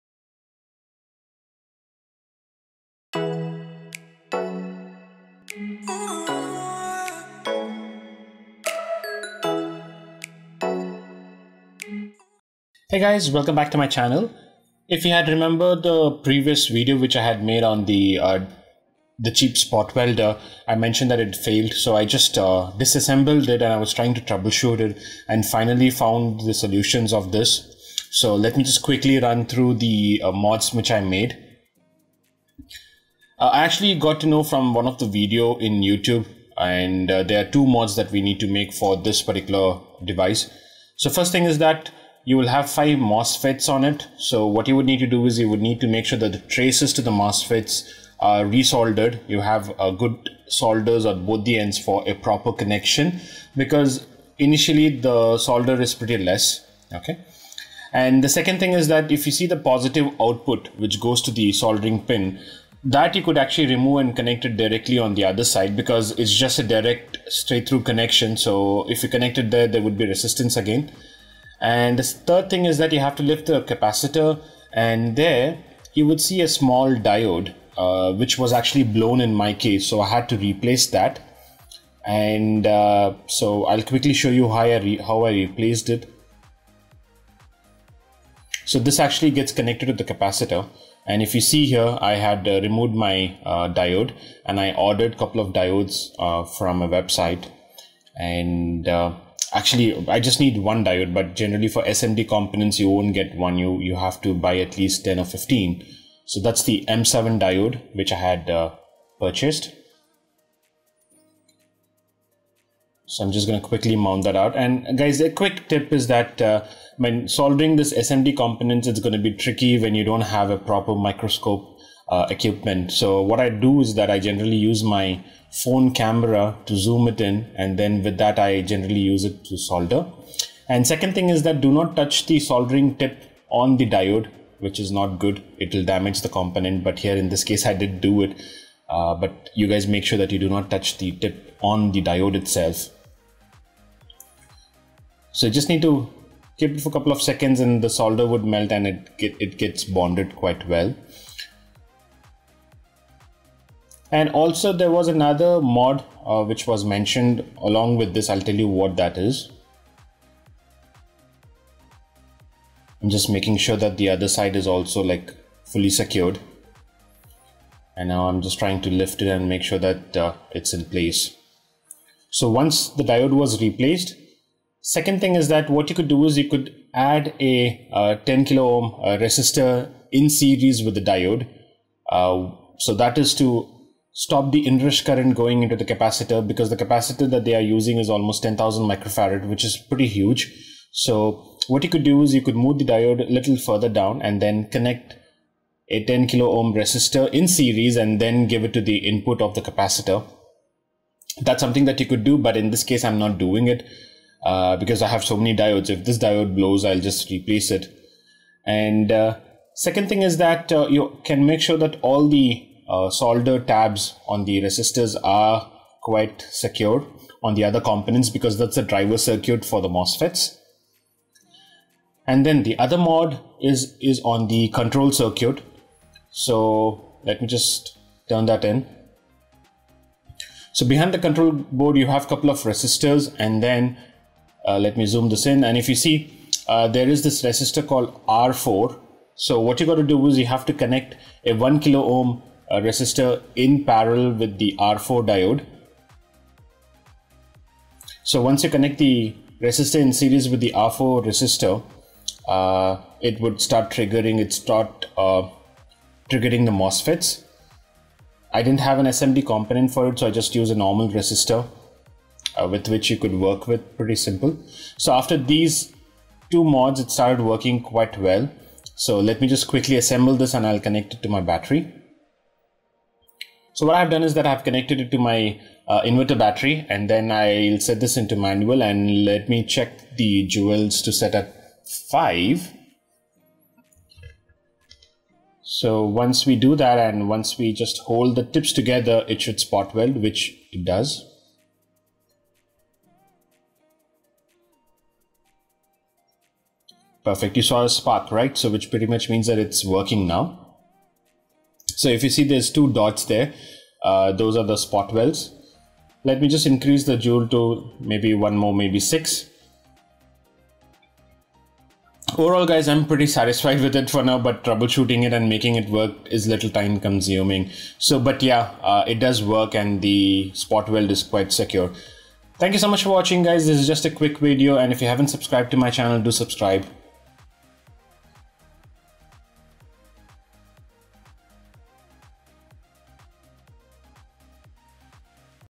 [FIXED] Cheap Battery Spot Welder | ordered form Banggood
When I got this product I was really disappointed, it was not functioning. After little bit of digging on the internet I have made a crude fix.
Ideally we need an inrush resistor in series with the diode mentioned in the video (which goes to the capacitor, but I have not added this to my fix). After that add a 1K ohm resistor in parallel on the control board R4 resistor.